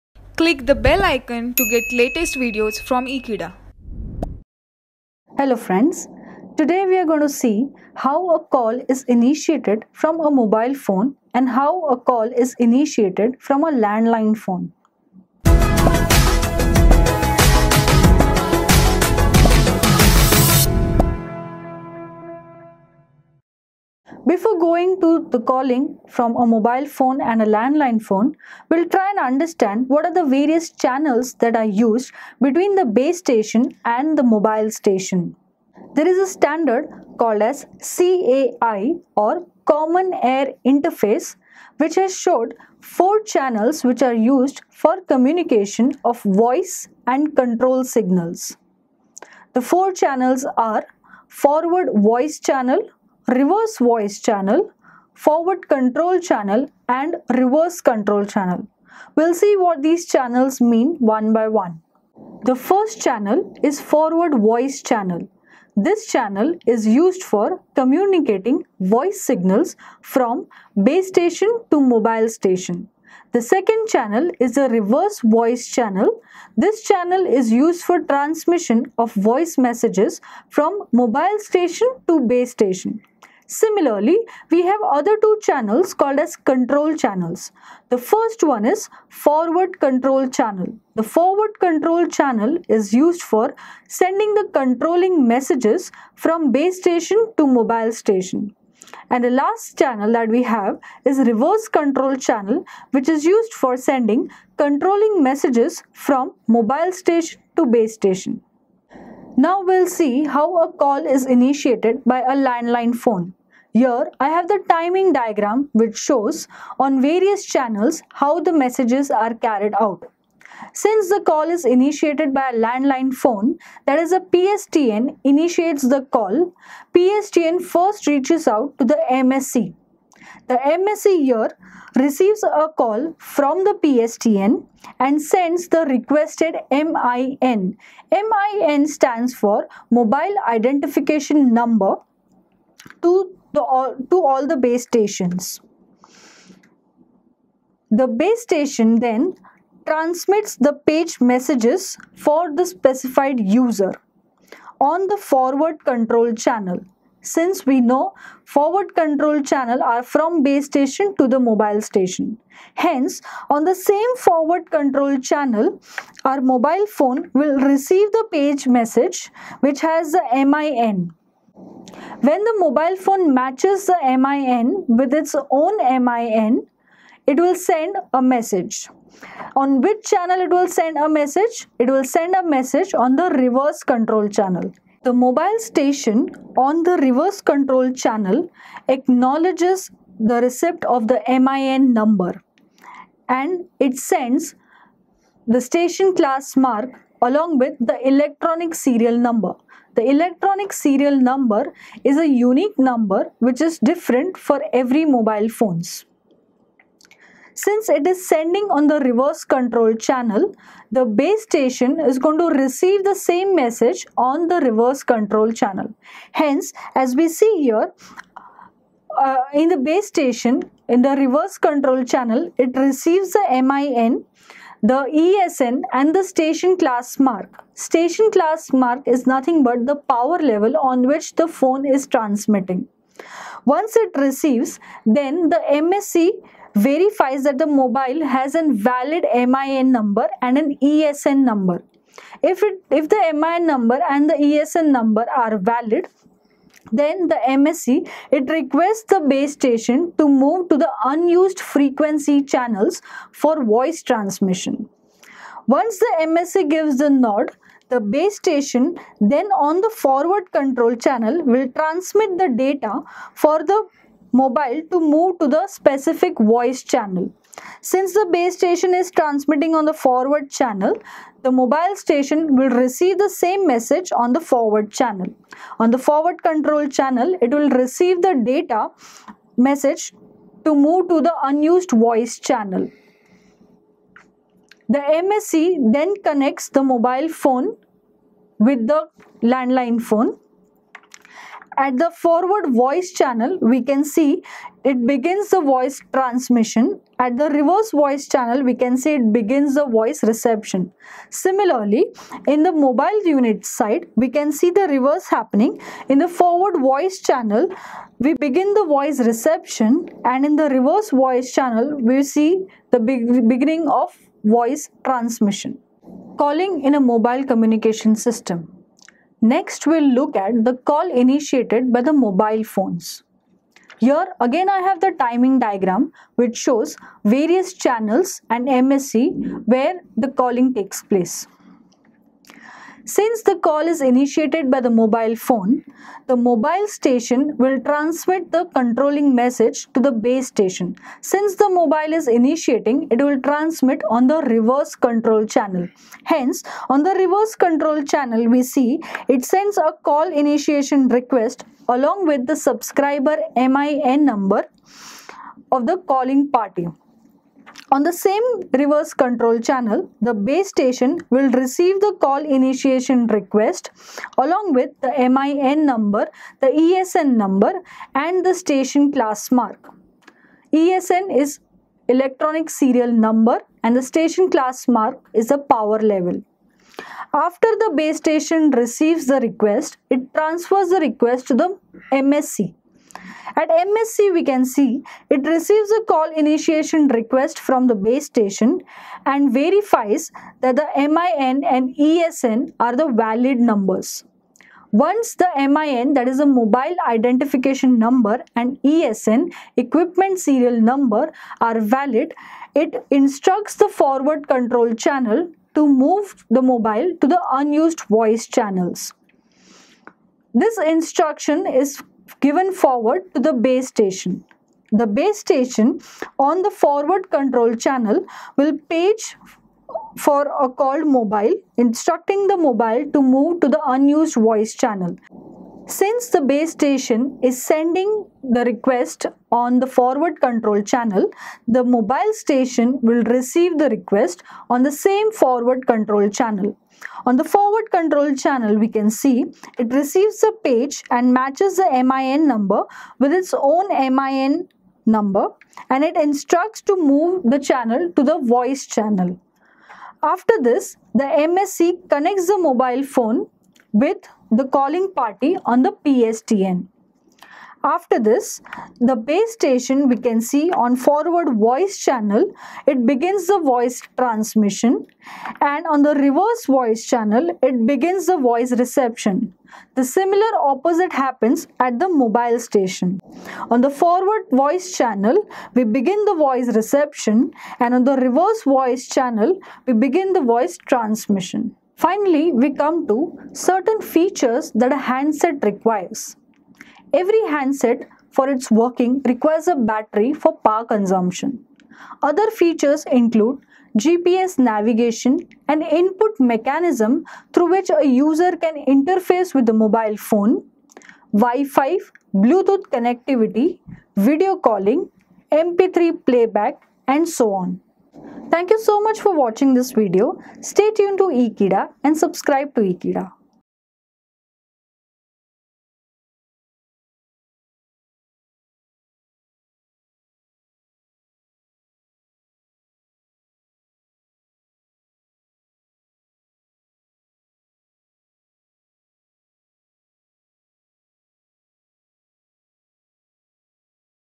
Calling in Mobile Communication - Fundamentals of Mobile Communication - Mobile Communication System
Subject - Mobile Communication System

Video Name - Calling in Mobile Communication

Chapter - Fundamentals of Mobile Communication

Faculty - Prof. Jeenal Rambhia

Happy Learning!